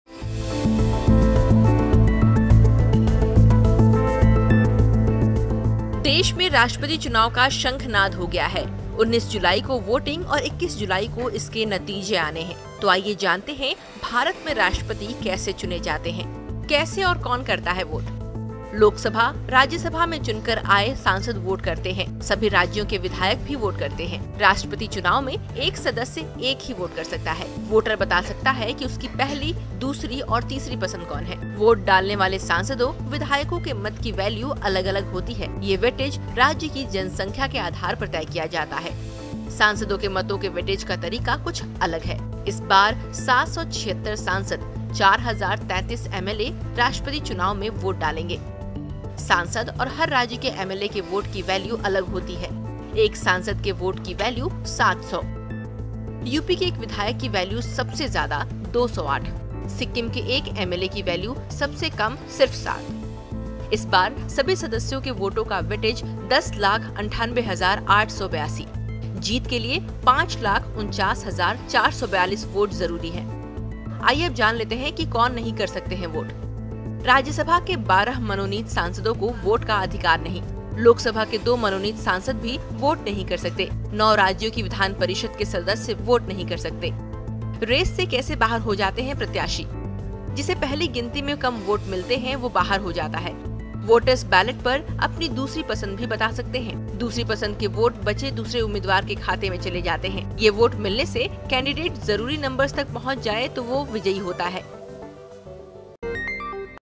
President Election: India में कैसे चुने जाते हैं राष्ट्रपति? आसान शब्दों में समझें प्रक्रिया..
राष्ट्रपति रामनाथ कोविंद (Ramnath Kovind) का कार्यकाल 24 जुलाई को खत्म हो रहा है. इससे पहले देश का अगला और 15वां राष्ट्रपति चुन लिया जाएगा. पिछले 45 साल से 25 जुलाई को निर्वाचित राष्ट्रपति कार्यभार संभालते रहे हैं.